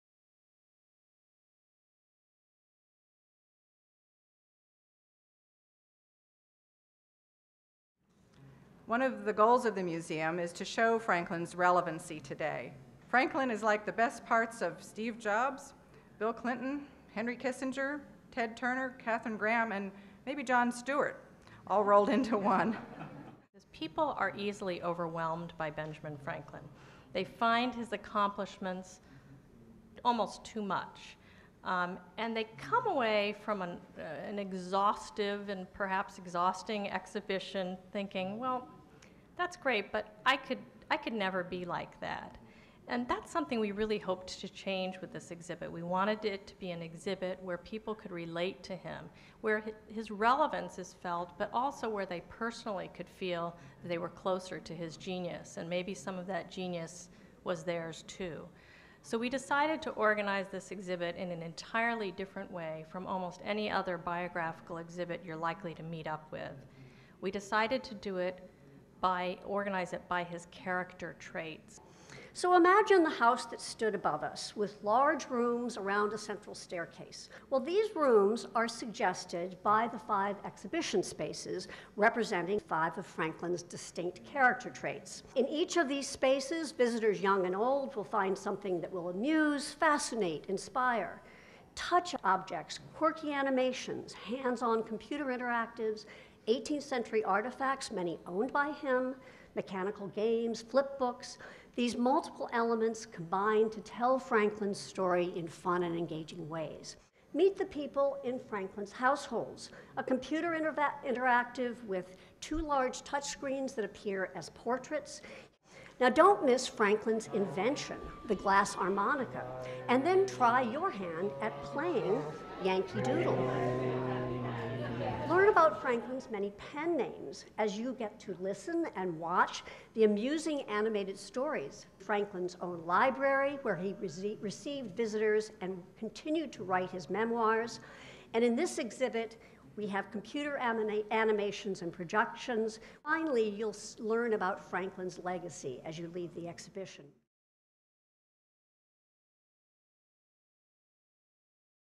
Benjamin Franklin Museum: An Exclusive First Look
The newly renovated Benjamin Franklin Museum in Old City Philadelphia is set to open on August 24, 2013. The museum's exhibit features quirky graphics, interactives, relics of his life, inventions and videos, all organized under five of Benjamin Franklin's personality traits.